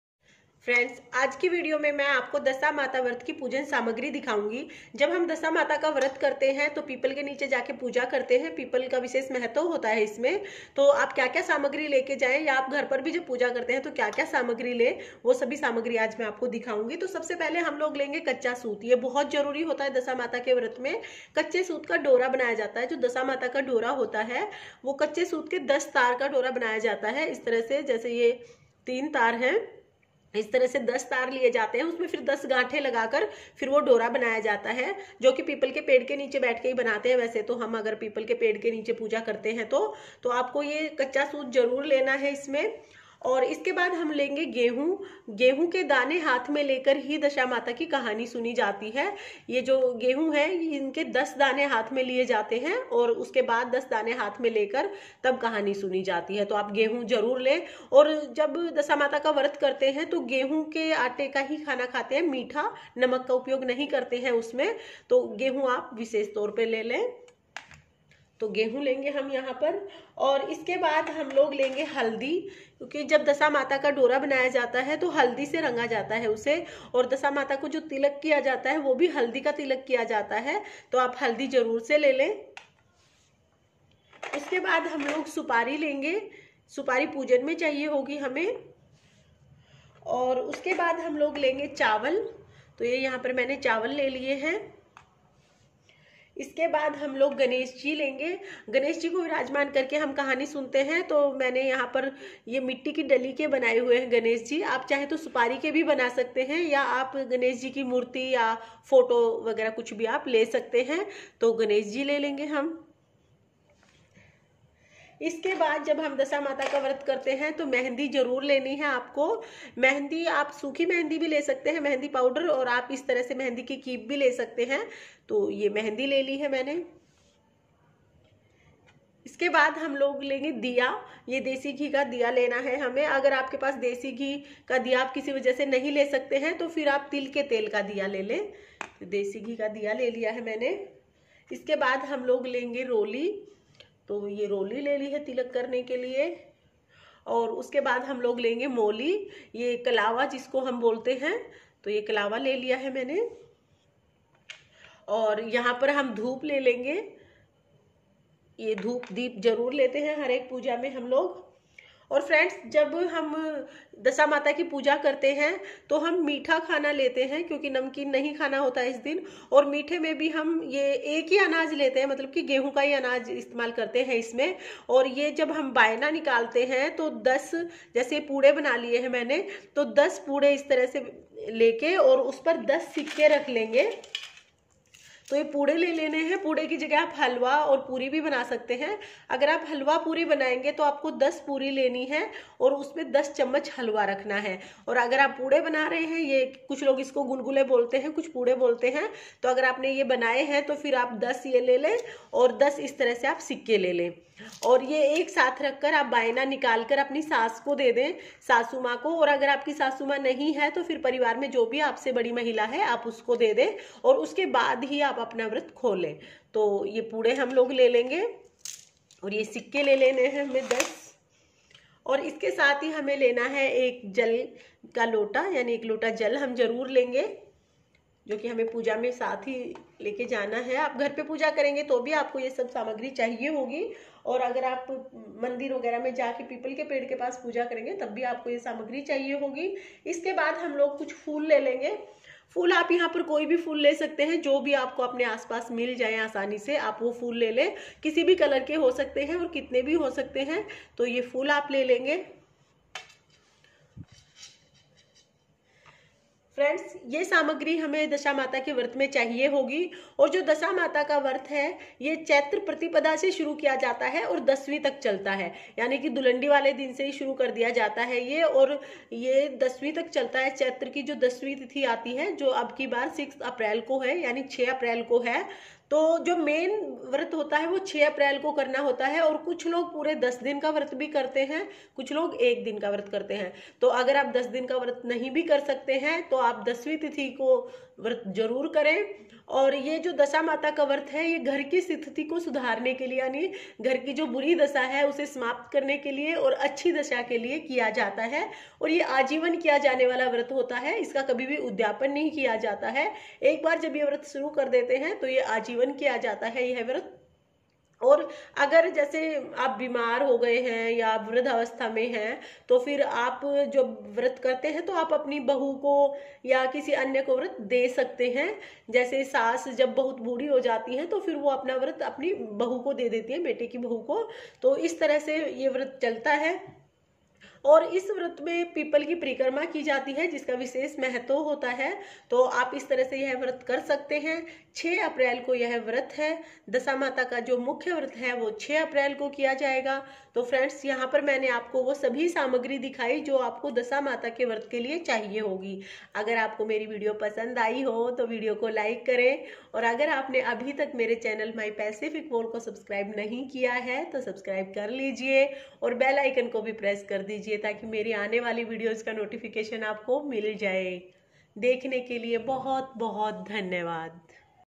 दशा माता व्रत सामग्री/दशा माता व्रत विधि |Dasha Mata vrat samagri/Dasha Mata vrat vidhi..
दशा माता व्रत की सामग्री/ दशा माता व्रत विधि |Dasha Mata vrat samagri /Dasha Mata vrat vidhi..

घर पर दशा माता की पूजा कैसे करें